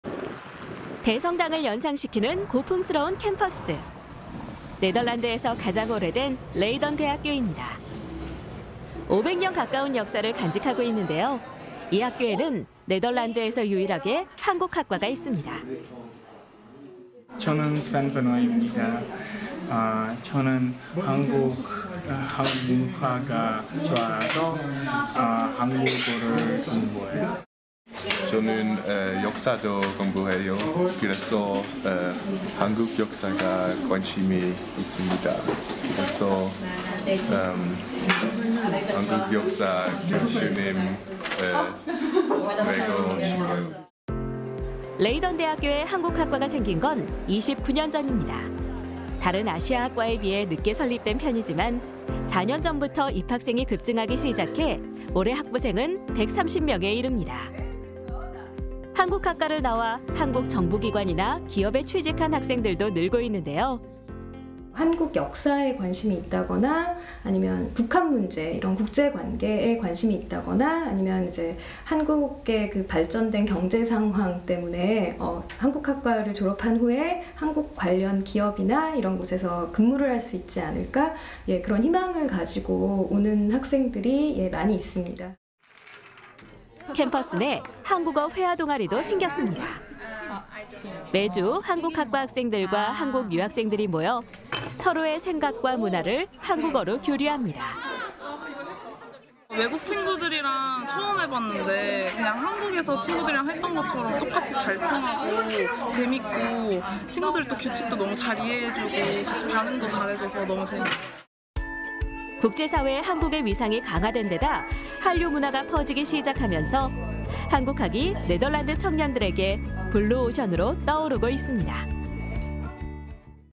한국학 인기 급상승…네덜란드의 한국학과 / YTN KOREAN
[앵커]
네덜란드 명문 대학교인 레이던대학교는 네덜란드에서 유일하게 한국학과가 있는 곳입니다.

이 한국학과의 인기가 급상승하면서 중국학과의 인기를 추월할 정도입니다.

한국을 공부하는 네덜란드 청년들을 장혜경 리포터가 만나봤습니다.

[기자]
대성당을 연상시키는 고풍스러운 캠퍼스.

네덜란드에서 가장 오래된 레이던대학교입니다.

5백 년 가까운 역사를 간직하고 있습니다.

레이던대학교에는 네덜란드에서 유일하게  한국학과가 있습니다.

[스벤 베노이 / 레이던대학교 한국학과 1학년 : 저는 스벤 베노이입니다. 저는 한국 문화가 좋아서 한국어를 공부하고 있어요.]

[아손 판 프로이옌 / 레이던대학교 한국학과 1학년 : 저는 역사를 공부해요. 그래서 저는 한국 역사에 관심이 있습니다. 그래서 한국 역사를 열심히 배우고 싶어요.]

레이던대학교에 한국학과가 생긴 건 29년 전입니다.

다른 아시아 학과에 비해 늦게 설립된 편이지만 4년 전부터 입학생이 급증하기 시작해 올해 학부생은 130명에 이릅니다.

한국학과를 나와 한국 정부기관이나 기업 등에 취직한 학생들도 늘고 있습니다.

[남애리 / 레이던대학교 한국학과 교수 : 한국 역사에 관심이 있다거나 북한 문제에 관심이 있다거나 한국의 발전된 경제 상황 때문에 한국학과를 졸업한 다음에 한국 관련 기업에서 근무할 수 있지 않을까는 희망으로 오는 학생들이 많이 있습니다.]

캠퍼스 내 한국어 회화 동아리도 생겼습니다.

매주 한국학과 학생들과 한국 유학생들이 모여 서로의 생각과 문화를 한국어로 교류합니다.

[김지민 / 레이던대학교 교환학생 : (윷놀이는) 외국 친구들이랑 처음 함께 해봤는데 한국에서 친구들이랑 했던 것처럼 똑같이 잘 통하고, 재밌고, 친구들도 (윷놀이) 규칙을 잘 이해하고 반응도 잘 해줘서 너무 재밌었어요.]

국제 사회 한국의 위상이 강화된 데다 한류 문화가 퍼지기 시작하면서 한국학이 네덜란드 청년들에게 블루오션으로 떠오르고 있습니다.

네덜란드 레이던에서 YTN 월드 장혜경입니다.